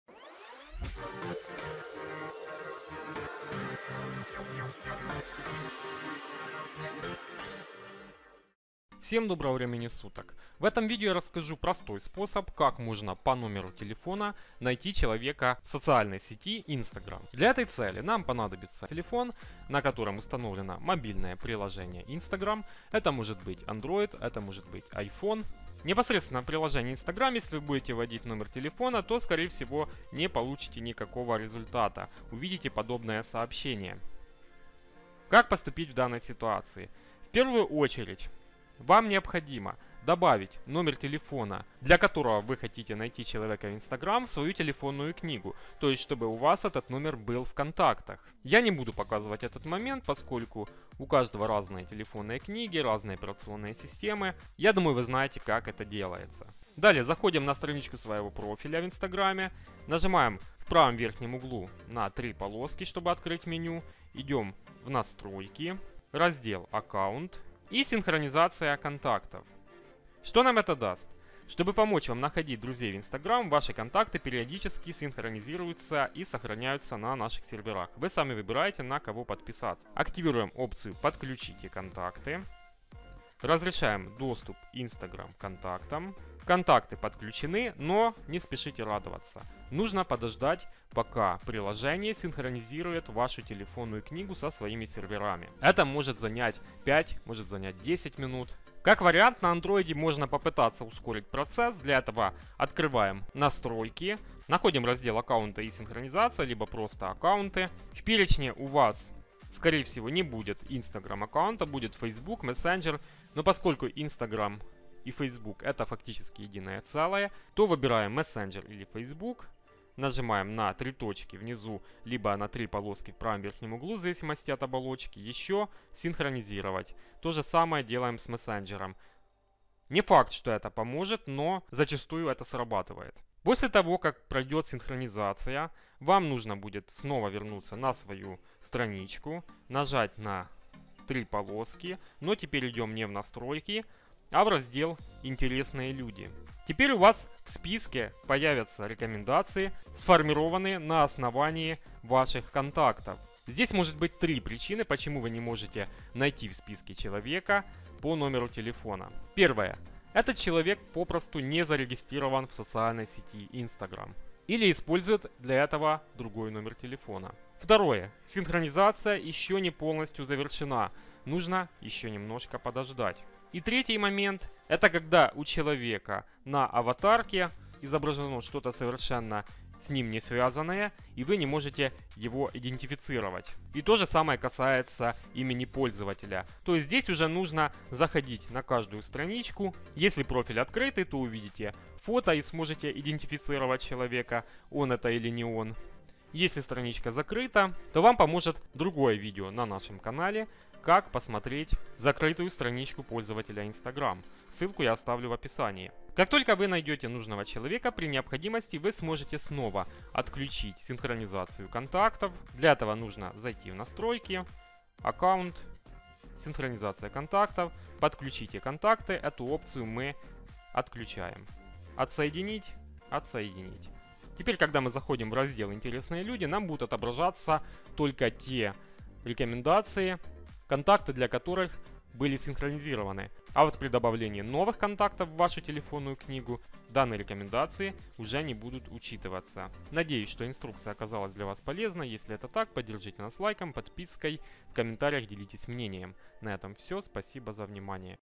Как в Инстаграме Найти Человека по Номеру Телефона 🔍 Поиск по Контактам в Instagram
Как в Инстаграме найти человека по номеру телефона? Делается это очень просто, не нужно устанавливать дополнительные приложения или использовать специальные сервисы.